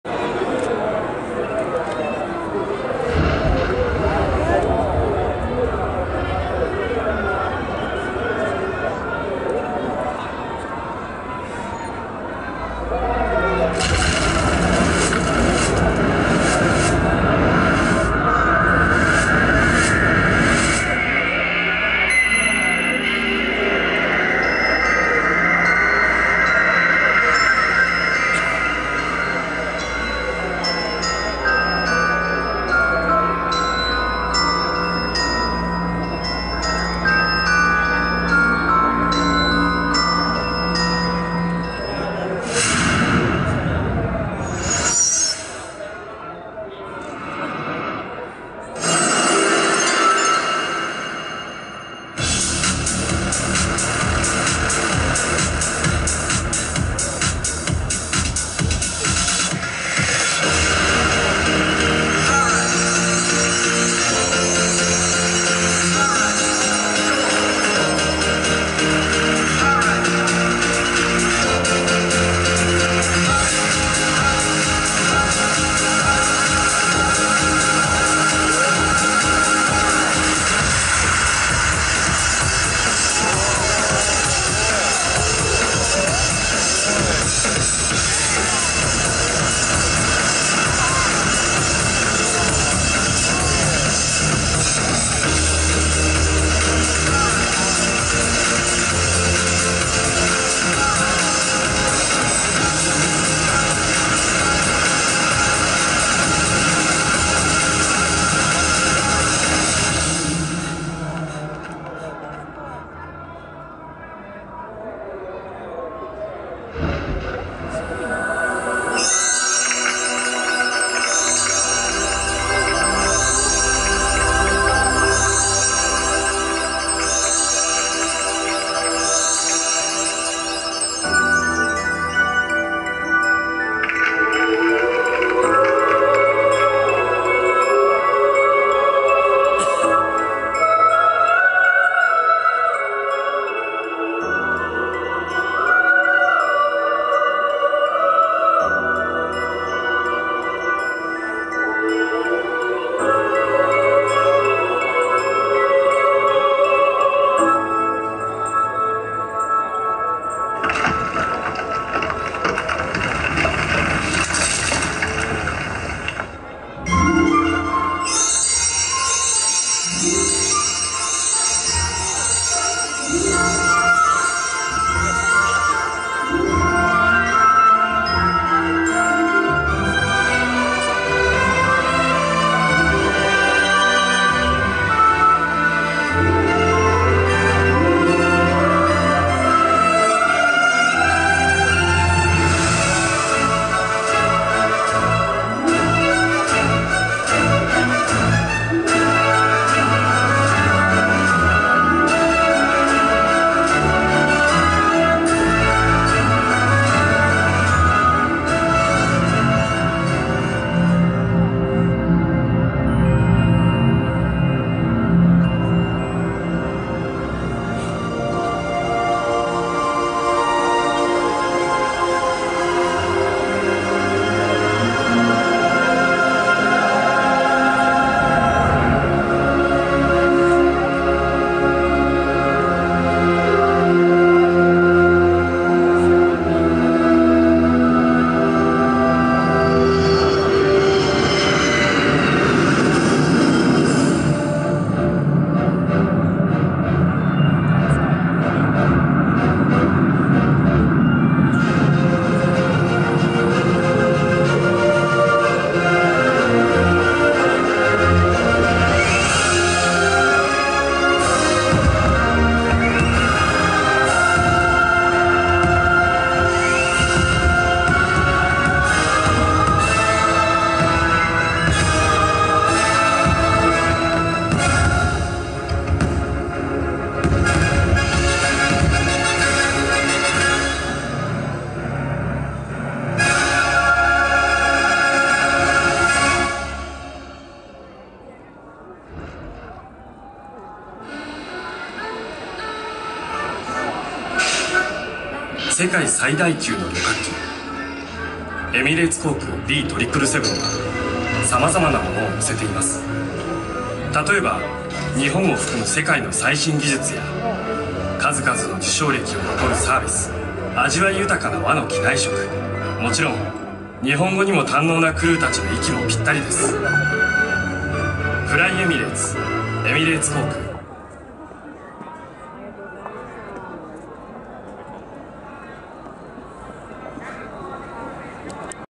よーせさん 光の絵本
2011大阪光のルネサンス 中央公会堂の光絵本です。
壁面が本当に動いているような錯覚が起きる面白い３D映像でした。

※手持ち撮影の為、ゆらゆらします＾＾；